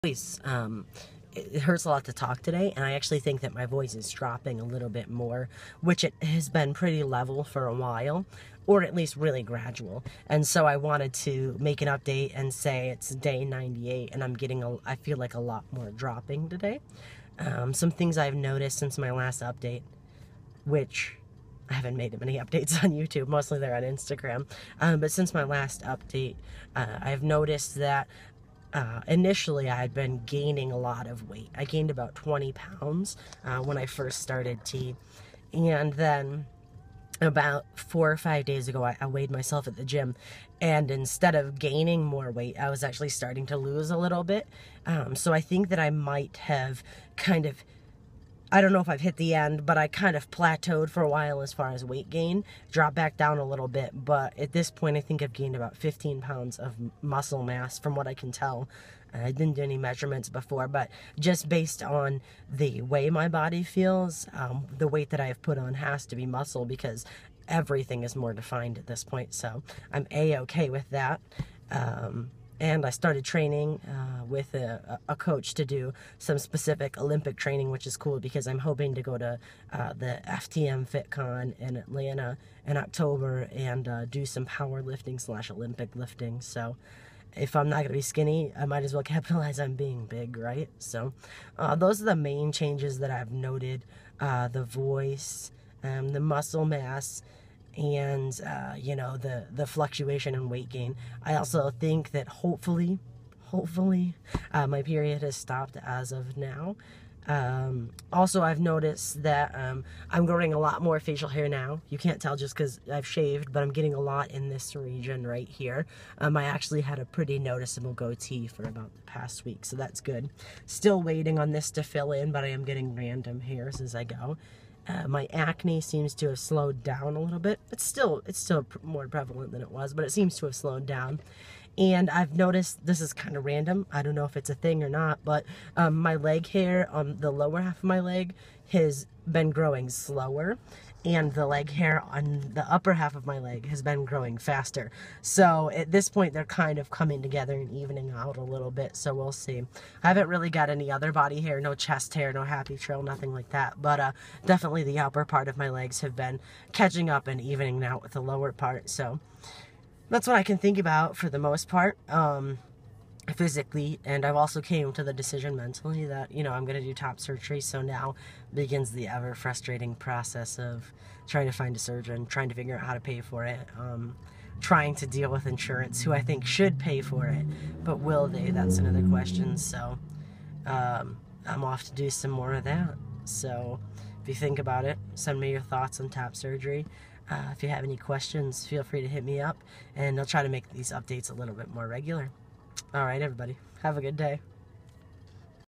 Day 98 on T voice update
Quick voice update and update on some on the t induced changes! Going to try to be more regular.